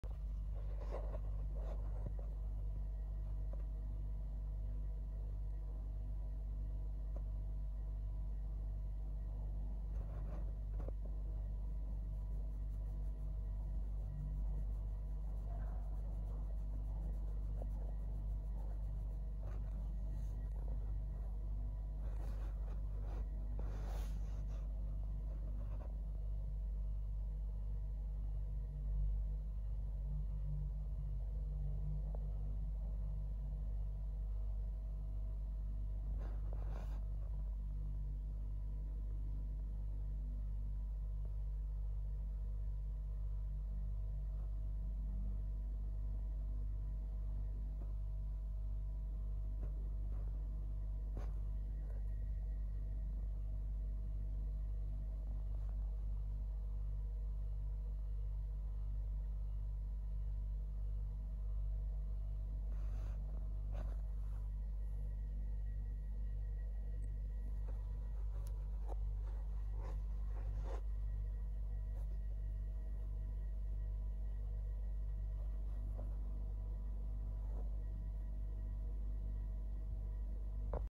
I'm Cute Gremlin, 4 months old - Stray Kitty Relaxing
Cute Kitten is Like: "What are you doing there? Are you going to give me some snack?"